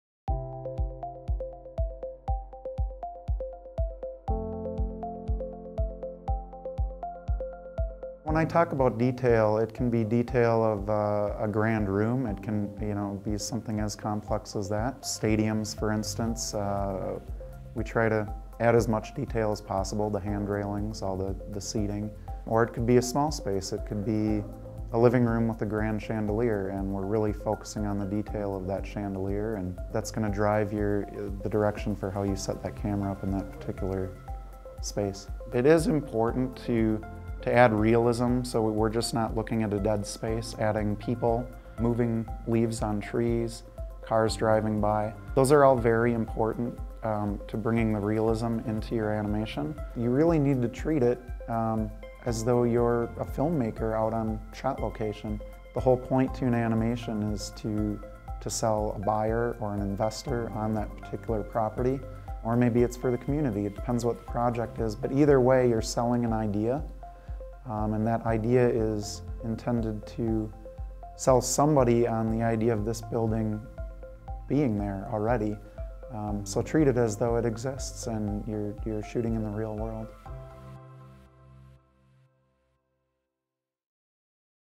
AU Explainers - Paying Attention to the Details with Christopher Lyner
Generating your standard fly-through animation doesn't cut it anymore in the architecture industry. Creating a captivating piece that will excite your client and captivate the audience requires some forethought. In this class we will go through an animation process, highlighting techniques used on a recent project that Tiltpixel was tasked with to sell an idea for a private school in South America. Using 3ds Max software we will go through setting up complex scenes, touching on lighting, rendering, and post producing.